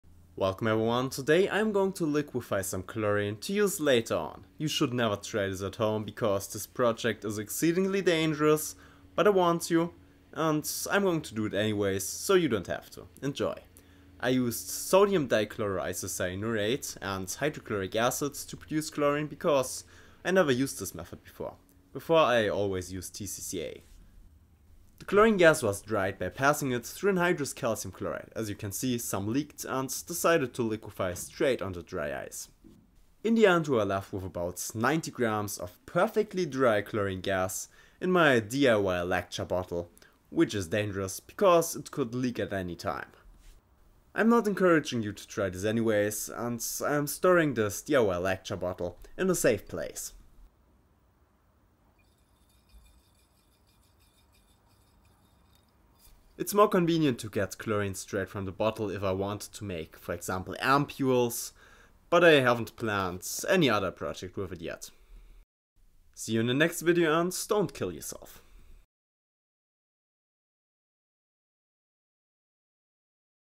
Thyzoids Chlorine Lecture Bottle
In this video I bottled up some chlorine for future use. This is incredibly dangerous so do not try this yourself. Steel is resistant to PERFECTLY DRY chlorine so the bottle will survive but it is still dangerous to store. The valve is made from chlorine resistant stainless steel. The pressure in this bottle is between 4-8 bars. Liquid chlorine has a density of about 1,56g/ml at its boiling point.